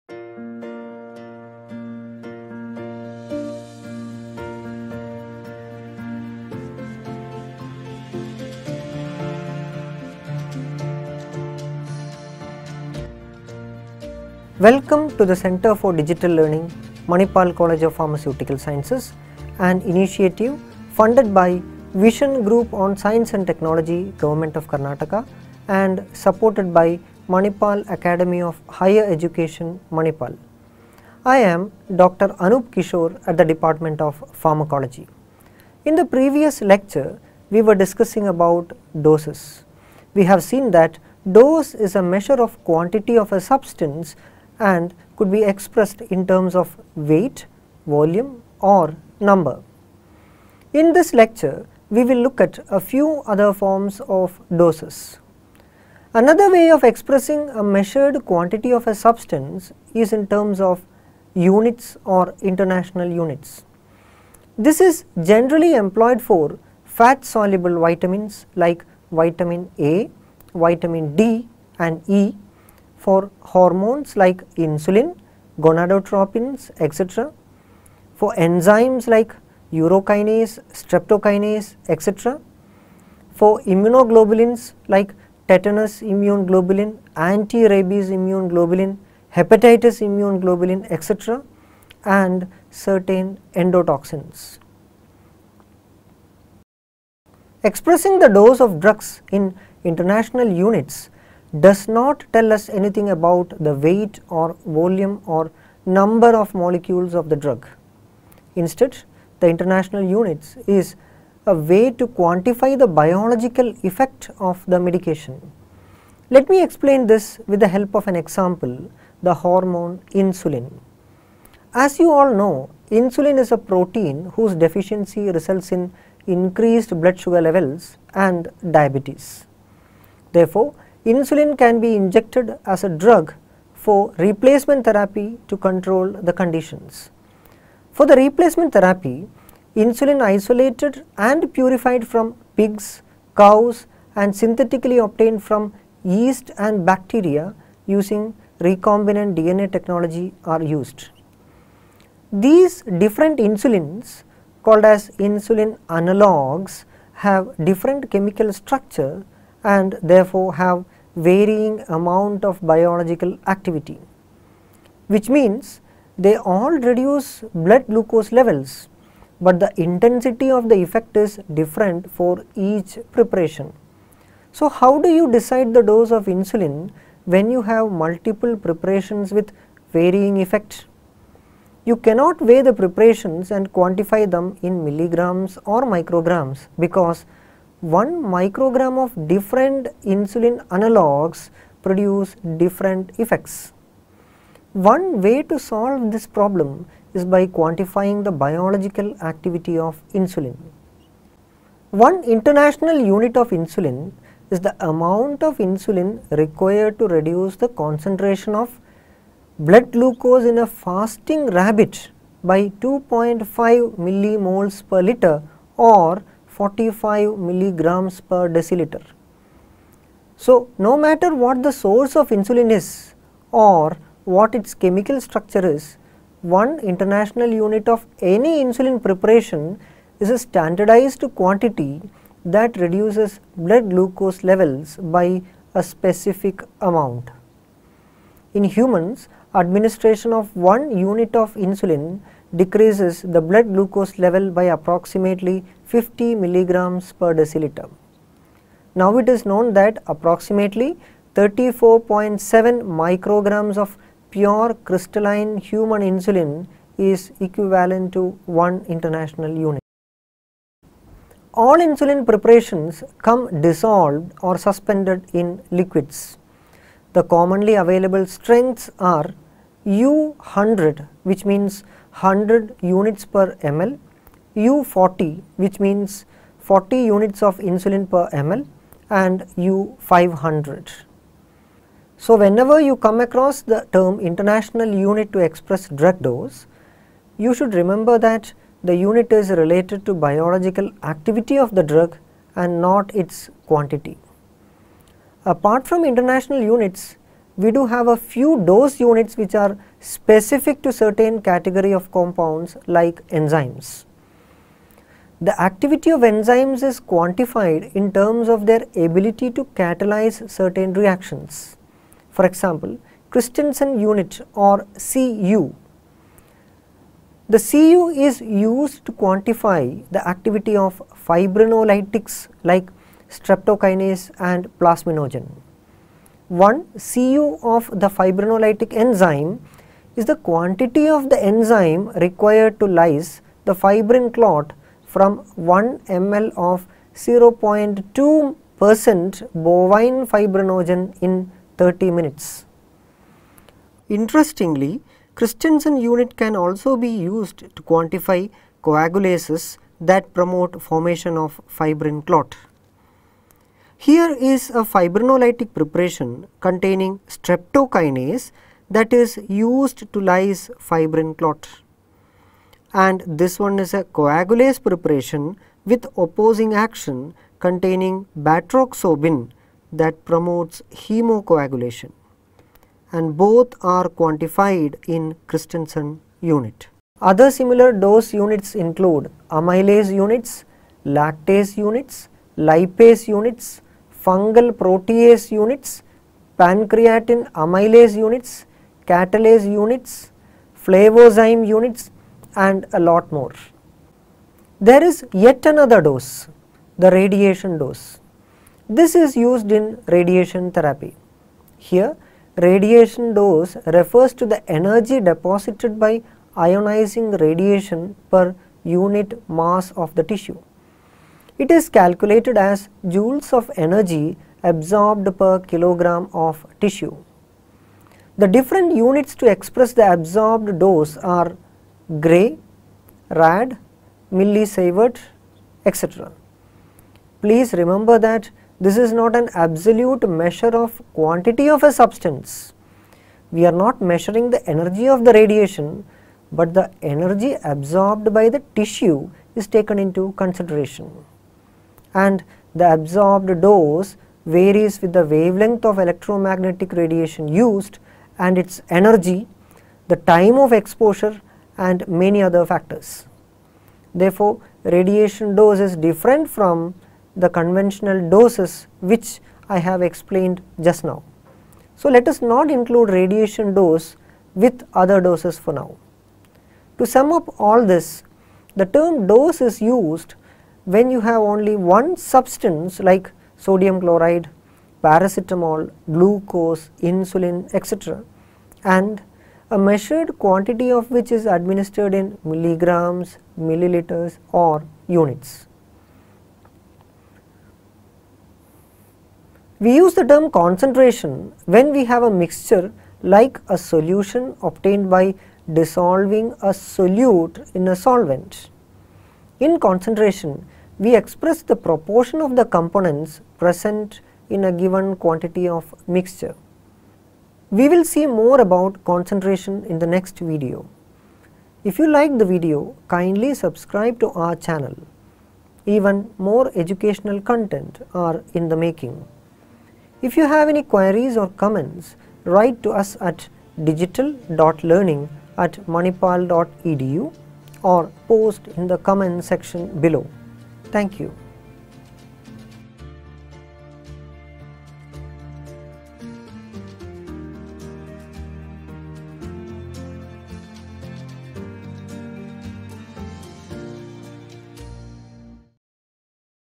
Lecture 02 - Dose (International Units)
A lecture series on Dose and Concentration, where the difference between dose and concentration is explained along with dose calculation and the different methods involved.

In Lecture 02 of this series, we are introducing the use of biological activity to quantify dose, expressed as International Units (IU).

Narration By:
Dr. Anoop Kishore

Video and Narration Team:
Dr. Jeyaprakash R.S.
Dr. Pawan G. Nayak
Mr. Manas Kinra

Credits:
Department of Pharmacology, Manipal College of Pharmaceutical Sciences, Manipal